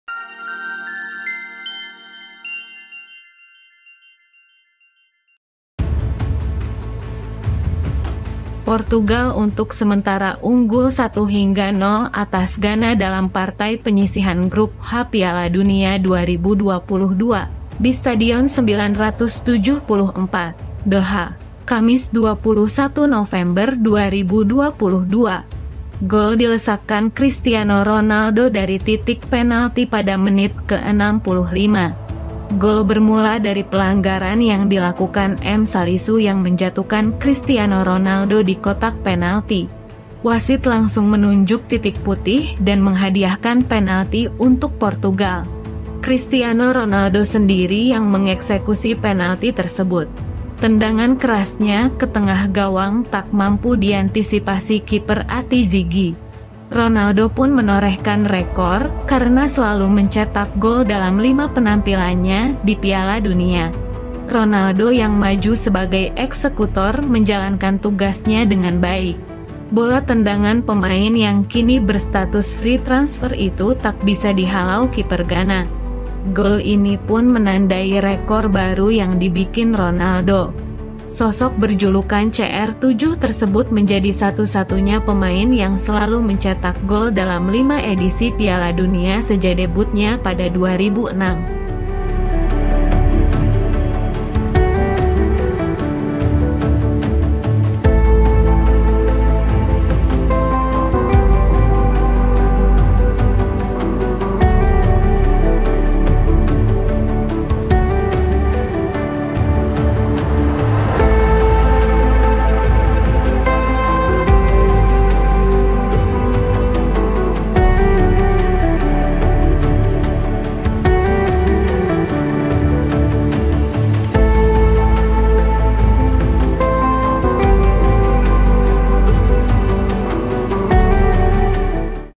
Ronaldo Cetak Rekor, Portugal Unggul 3-1
Portugal untuk sementara unggul 3-1 atas Ghana dalam partai penyisihan Grup H Piala Dunia 2022, di Stadion 974, Doha, Kamis (21/11/2022). Gol dilesakkan Cristiano Ronaldo dari titik penalti pada menit ke-65.

Gol bermula dari pelanggaran yang dilakukan M Salisu yang menjatukan Cristiano Ronaldo di kotak penalti. Wasit langsung menunjuk titik putih dan menghadiahkan penalti untuk Portugal.

Cristiano Ronaldo sendiri yang mengeksekusi penalti tersebut. Tendangan kerasnya ke tengah gawang tak mampu diantisipasi kiper Ati Zigi. Ronaldo pun menorehkan rekor karena selalu mencetak gol dalam lima penampilannya di Piala Dunia.

Ronaldo yang maju sebagai eksekutor menjalankan tugasnya dengan baik